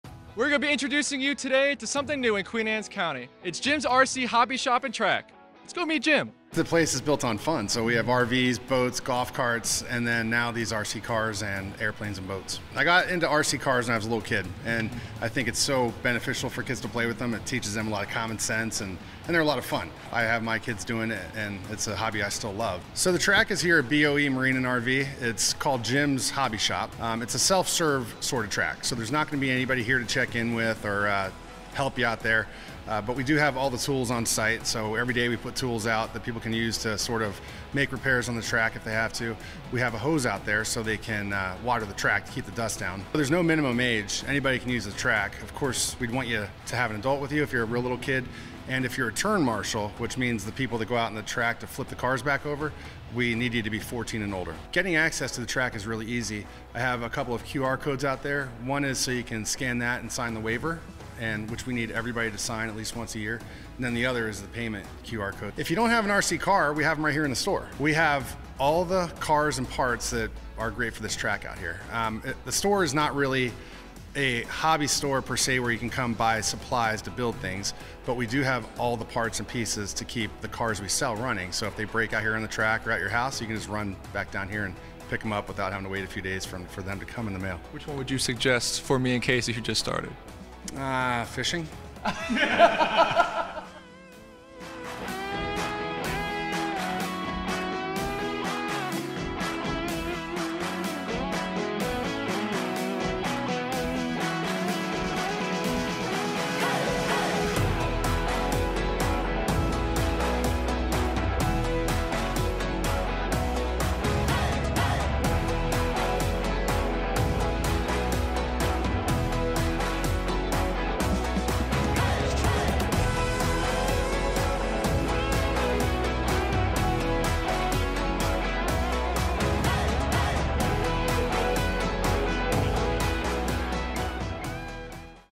Jim's RC Hobby Shop
We'd like to introduce you to something new and fun in Queen Anne's County... It's Jim's RC Hobby Shop in Stevensville!

They are located at 325 Cleat Street, right inside BOE Marine. Check them out!